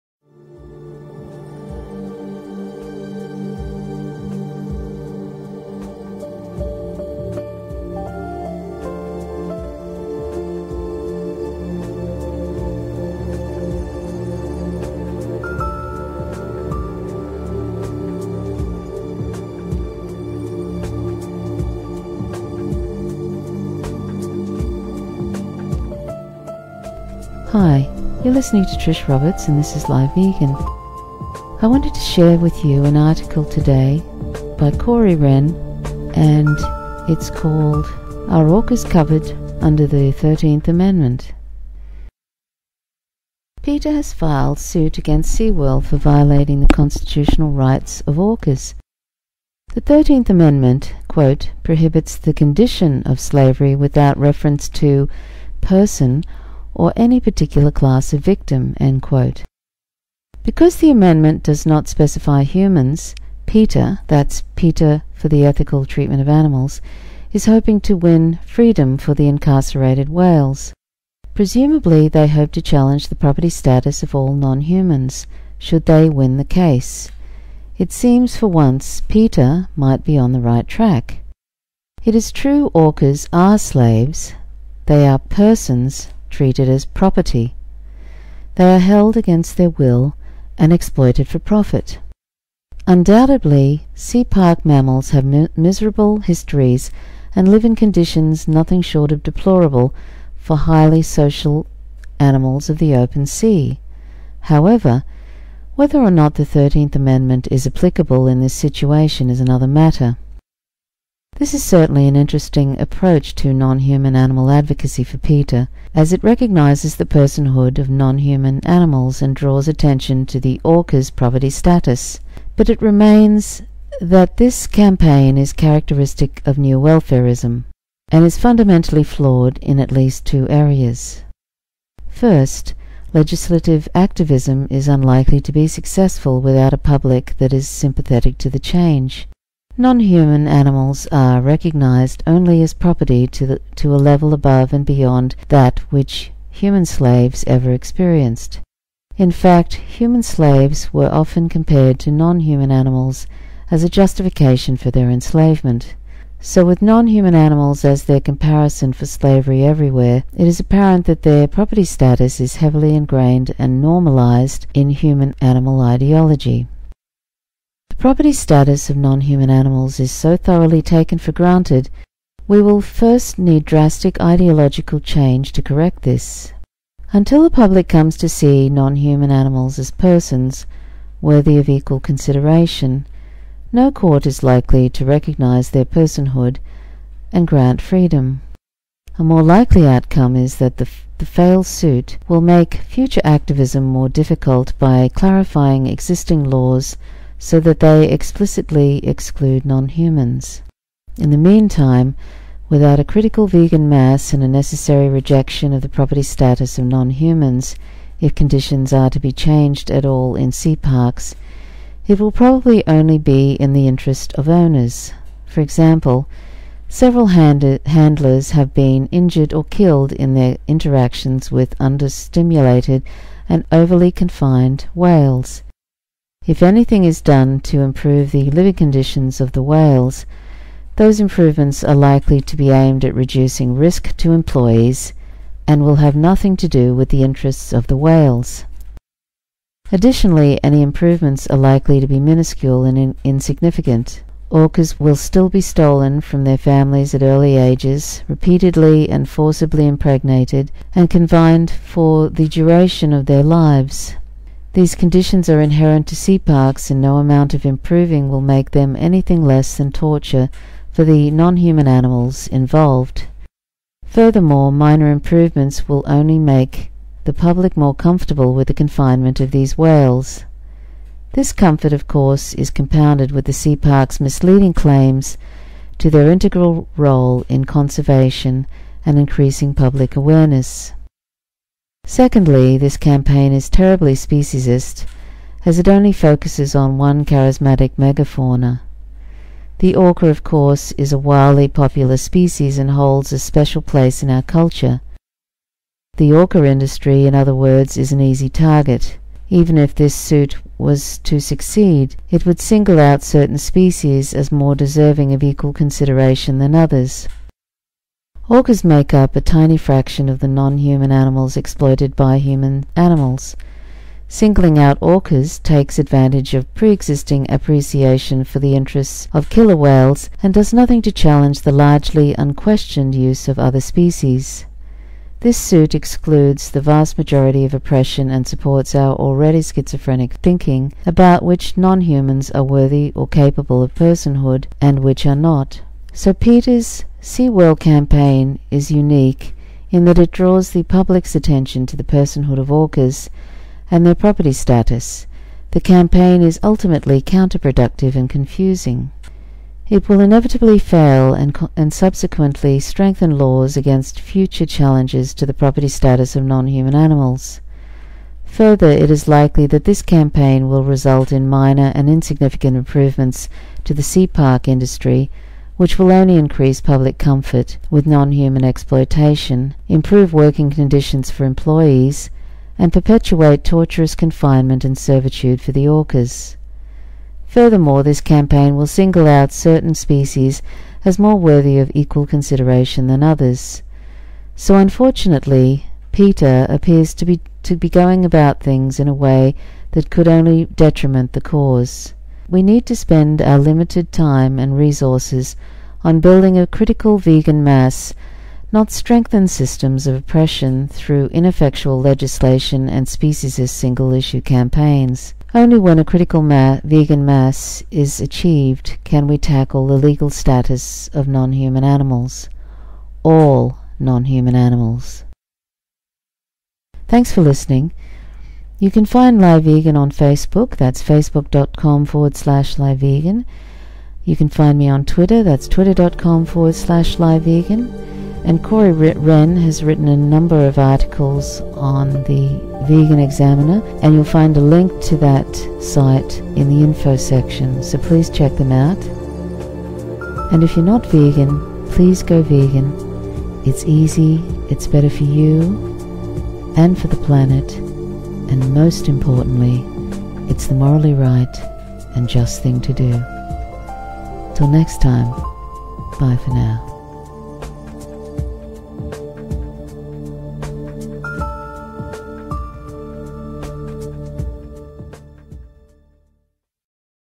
LiveVegan Podcast #24: "Are orcas covered under the 13th amendment?"
LiveVegan Podcast on article by Corey Wrenn
Are orcas covered under the 13th amendment? - Roanoke vegan | Examiner.com